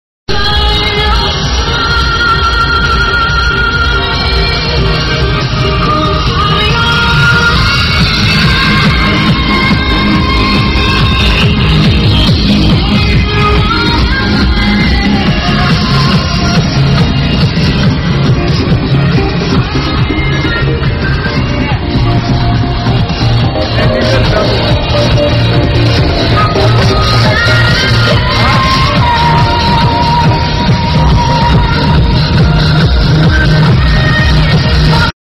Preparativi Stand Motoraduno Scalea 2009
16°Motoraduno Scalea...
Il The Pearl non poteva di certo mancare a questo Straordinario Evento...E come sempre Pronto a Divertirsi e far divertire a Tutti...